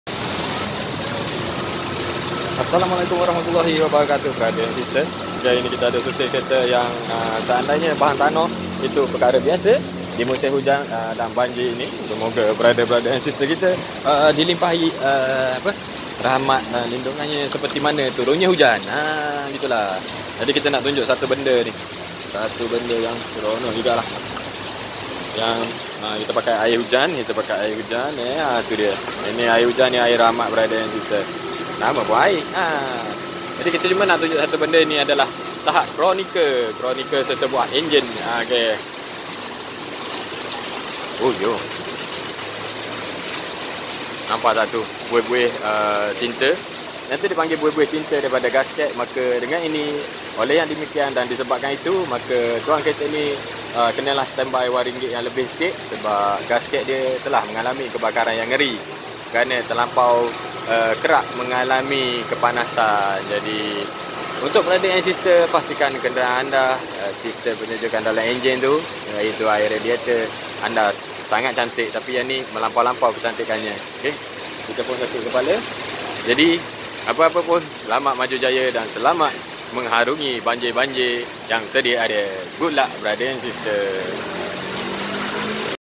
salah satu punca enjin overheat..
ini ada lah contoh bahawa berlaku nya kepanasan pada bahagian enjin
.. kewujudan buih2 halus x halus ini adalah disebabkan gasket head mengalami kerosakan ataupun dh terbakar.. utk pencegahan awalan terdapat sesetengah produk dipasaran mampu membendung kejadian ini dri lebih parah.. seandainya semakin parah.. cara rawatan adalah berjumpa pakar y berhampiran.. pastikan brader n sister mempunyai bajet y mencukupi ..👌👍💪🤏 good luck brader n sister..